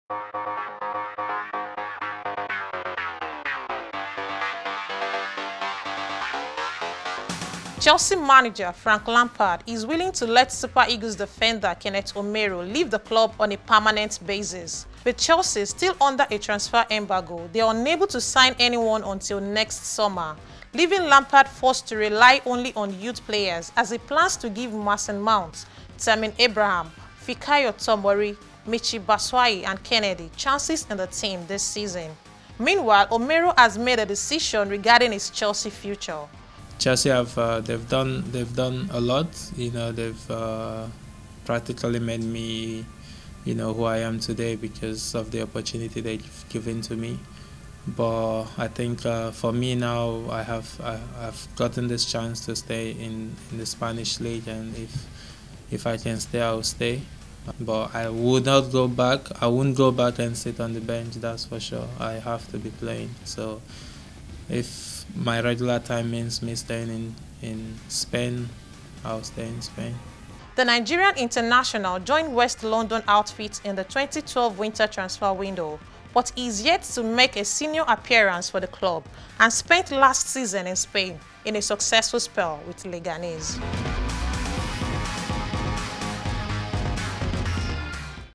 Chelsea Loanee, Omeruo, Opts out of Lampard's Squad
Super Eagles defender and Chelsea loanee, Kenneth Omeruo, insists he won't sit on the bench under new manager, Frank Lampard. Omeruo, who had a blissful season with Leganese FC in the Spanish La Liga last season, wants to enjoy full-time play in the field of action.